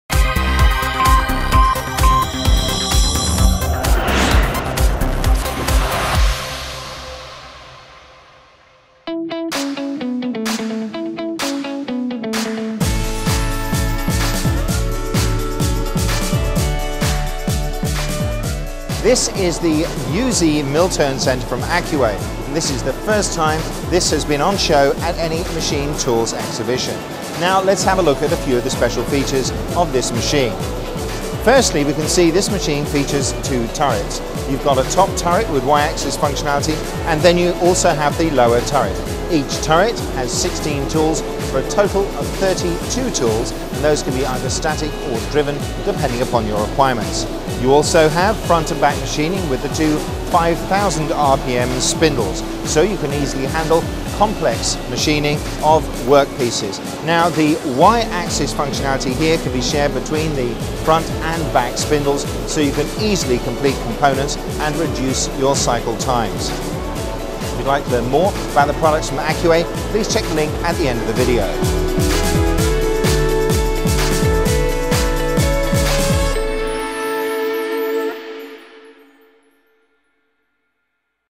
MTS - Accuway - Mill Turn Center - 2015 TIMTOS
For more information on Taiwan machine tool related news and exhibition please

All of Accuway’s machines are made using very high quality components and at TIMTOS 2015 we take a quick look at one of their mill turn centers in their UZ series.

This machines features two turrets and 2 spindles for both front and back machining capabilities. With up to 32 tools this machine can efficiently handle complex component machining.